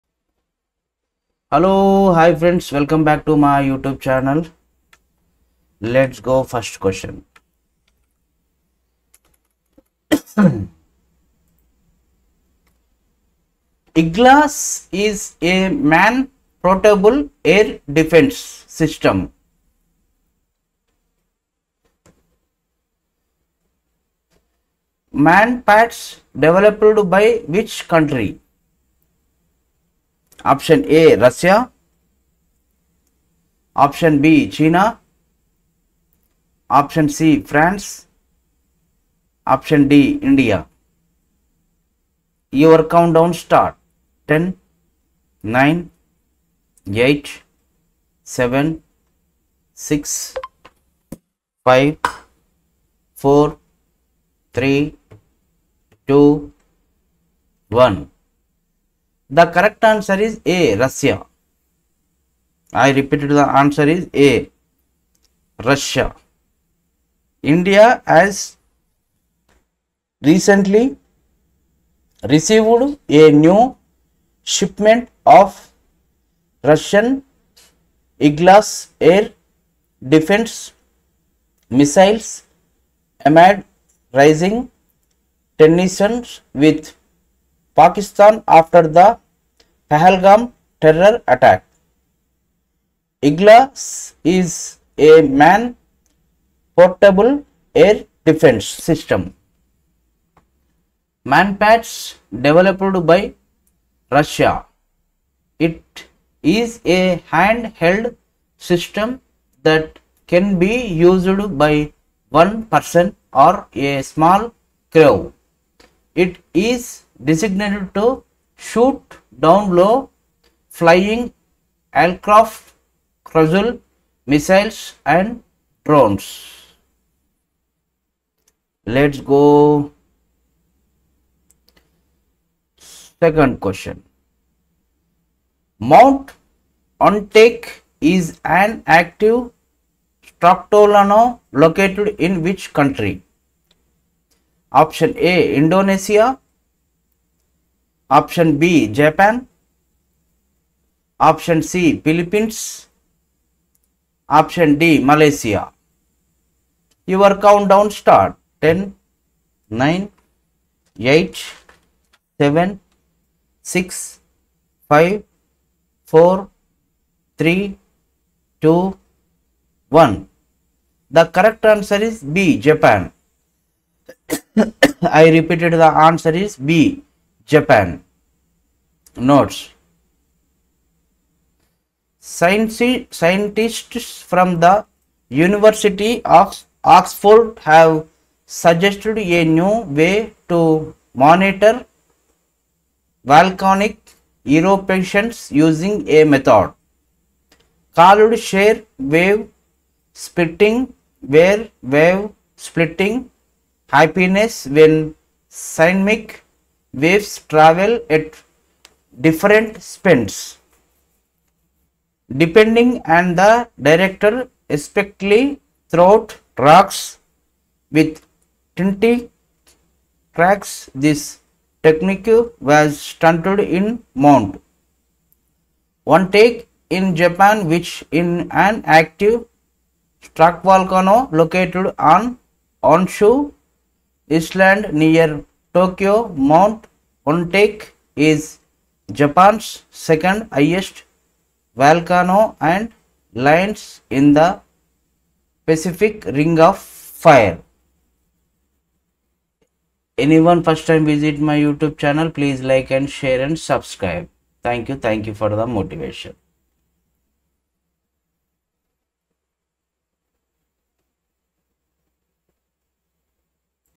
Igla-S is a Man Portable Air Defence System (MANPADS) developed by which country?
[A] Russia
[B] China
[C] France
[D] India
Hide Answer

Correct Answer: A [Russia]
Notes:
India has recently received a new shipment of Russian Igla-S air defence missiles amid rising tensions with Pakistan after the Pahalgam terror attack. Igla-S is a Man Portable Air Defence System (MANPADS) developed by Russia. It is a hand-held system that can be used by one person or a small crew. It is designed to shoot down low-flying aircraft, cruise missiles, and drones.